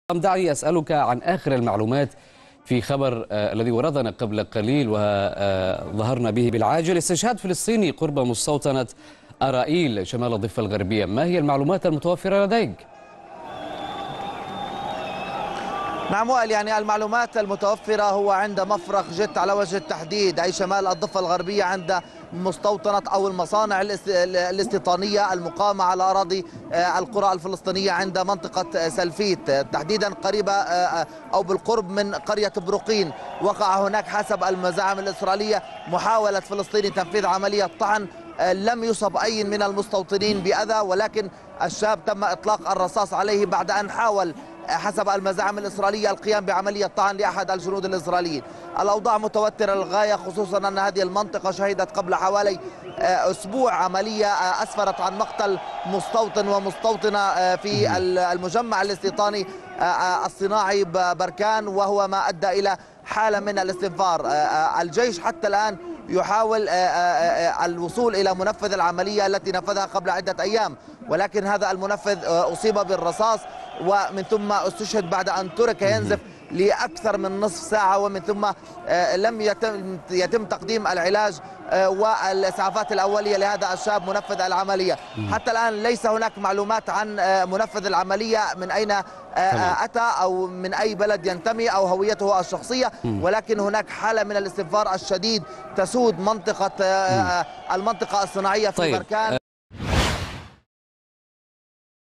مراسل الغد: استشهاد فلسطيني قرب مستوطنة أرئيل شمال الضفة الغربية
أفاد مراسل الغد باستشهاد فلسطيني برصاص جيش الاحتلال الإسرائيلي قرب مستوطة أرئيل شمالي الضفة الغربية بزعم محاولته تنفيذ عملية طعن.
المزيد من التفاصيل مع ضياء حوشية مراسل قناة الغد خلال مشاركته مع الإعلامي وائل العنسي.